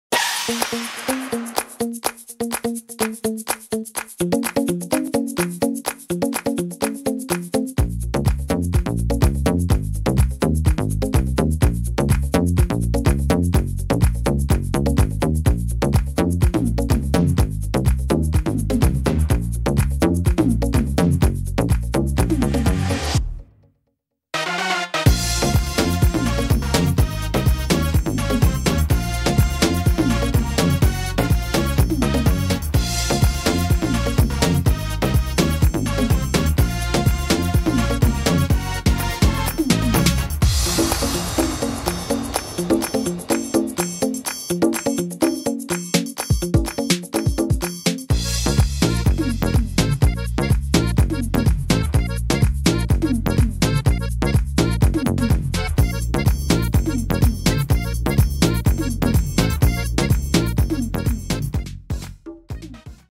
Splatter Painting - craft for kids
This is an easy painting technique for literally hundreds of different creations - if it needs painting, you can splatter it! It's great fun for kids - but make sure you protect surfaces and clothes!!! Have fun!